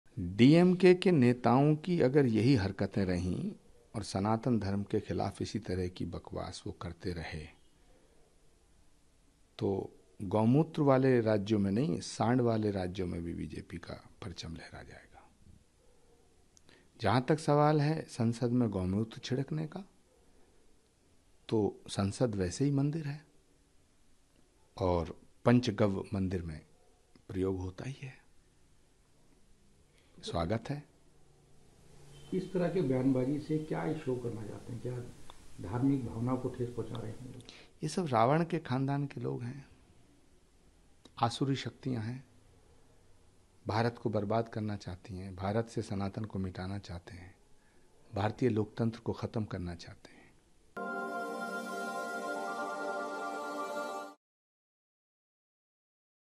“If DMK leaders continue to behave like this then…” Acharya Pramod amid 'Gaumutra’ remark row
Reacting sharply to the 'Gaumutra States' jibe at Bharatiya Janata Party made by DMK MP DNV Senthilkumar S, Congress leader Acharya Pramod Krishnam said if such statements continue to come from their end, the BJP might emerge victorious in the states with ‘Saand’. "If DMK leaders continue to behave like this and keep talking like this against Sanatan Dharma, then BJP's flag will be hoisted not only in 'Gaumutra' states but also in states with 'saand',” said Acharya Pramod Krishnam.